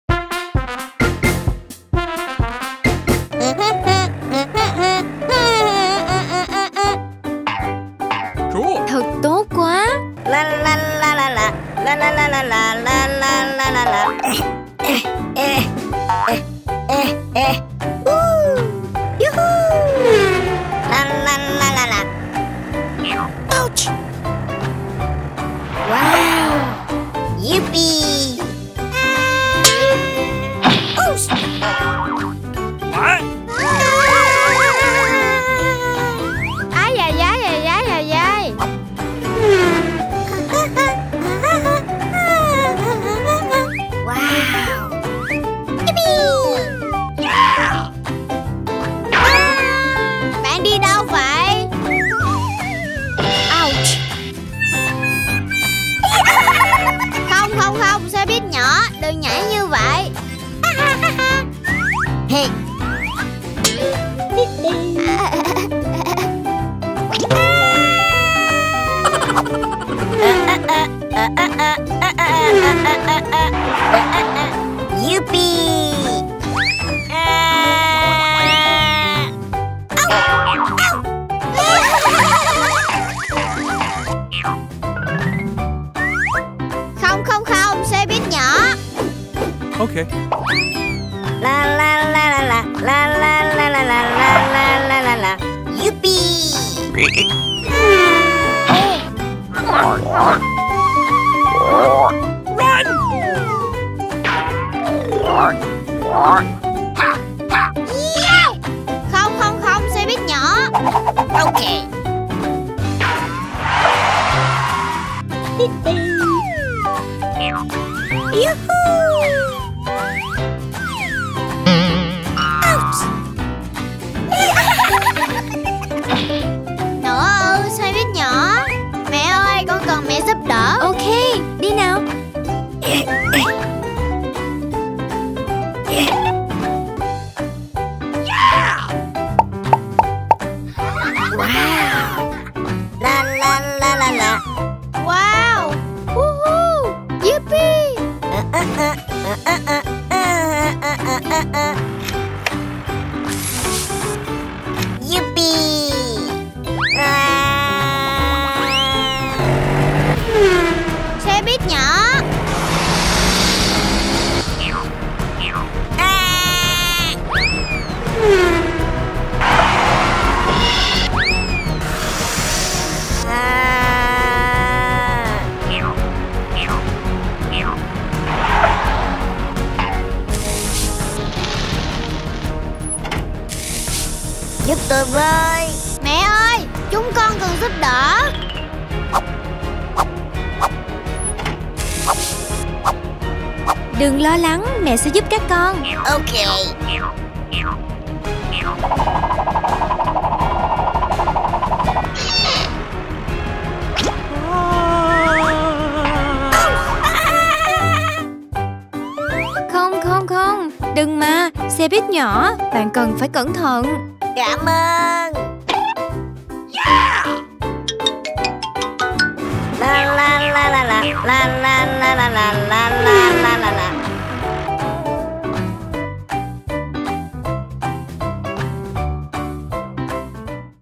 Cuộc phiêu lưu của xe buýt đồ chơi Vlad và Mom
Một chiếc xe buýt đồ chơi nhỏ cưỡi và vui chơi trong sân chơi. Vlad và mẹ giúp đồ chơi ra khỏi hố cát.